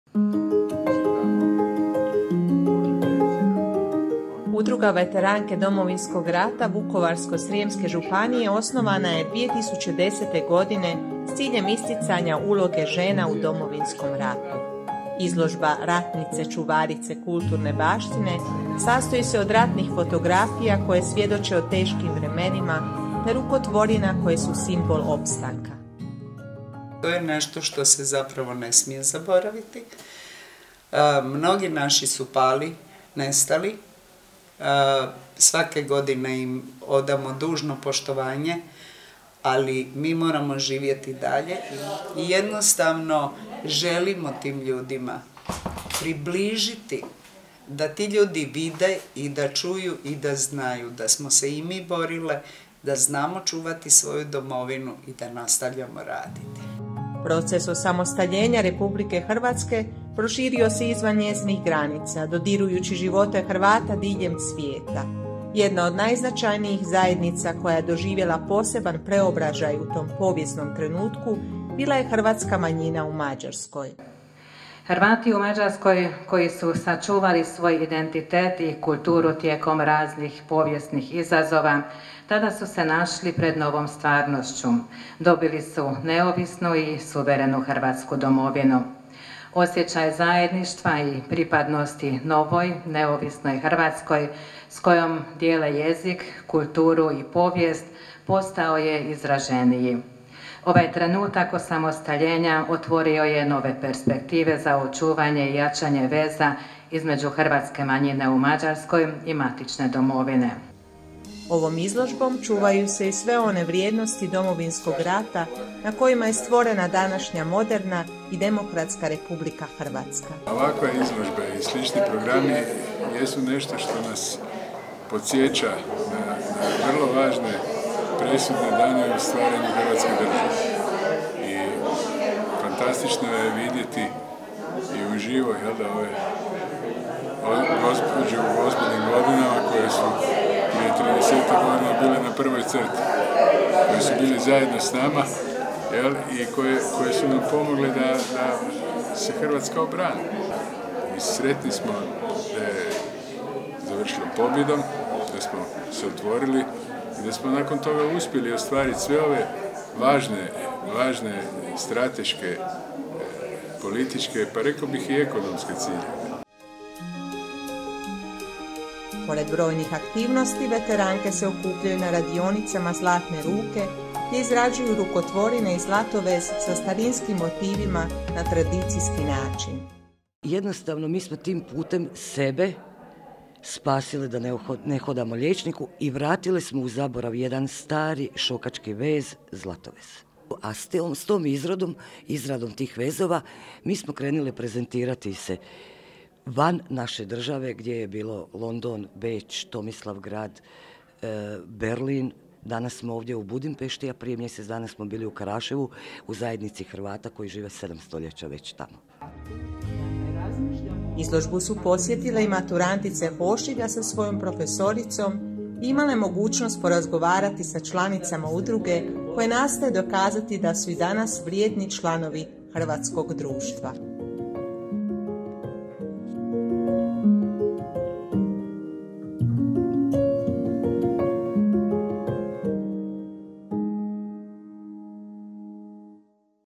Snažne čuvarice identiteta
U povodu obilježavanja Dana sjećanja na žrtve Domovinskog rata i Dana sjećanja na žrtvu Vukovara i Škabrnje, u prostorijama Croatice otvorena je izložba ratnih fotografija, rukotvorina i zlatoveza pod nazivom „Ratnice čuvarice kulturne baštine“, s kojom su se hrvatskoj zajednici u Budimpešti predstavile članice Udruge Veteranke Domovinskog rata Vukovarsko-srijemske županije.

Snimatelj: Zsolt Kardos
Reporterka: Čarna Kršul
Produkcija: Timea Šakan-Škrlin, ravnateljica Croatice